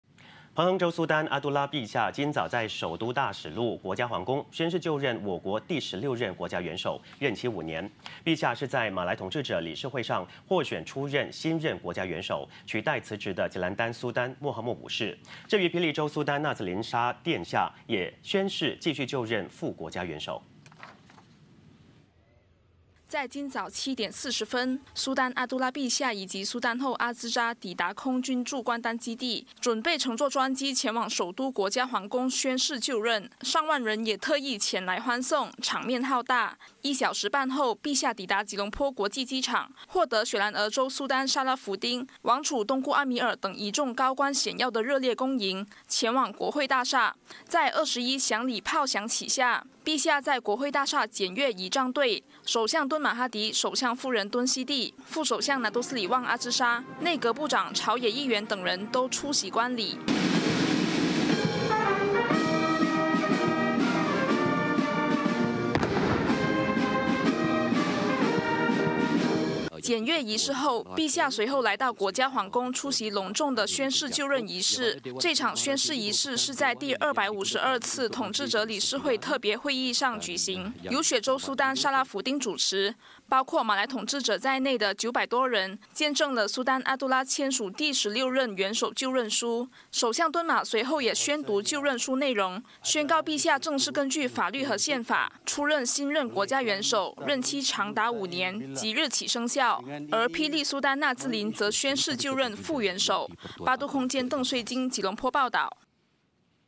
彭亨苏丹阿都拉今宣誓 就职第16任国家元首
彭亨州苏丹阿都拉陛下今早在首都大使路国家皇宫宣誓就任我国第16任国家元首，任期5年。陛下是在马来统治者理事会上，获选出任新任国家元首，取代辞职的吉兰丹苏丹莫哈末五世。至于霹雳州苏丹纳兹林沙殿下也宣誓继续就任副国家元首。